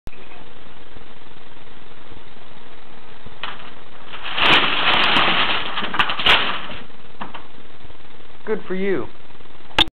building fail
i was at my friends house and he touched it and it FELL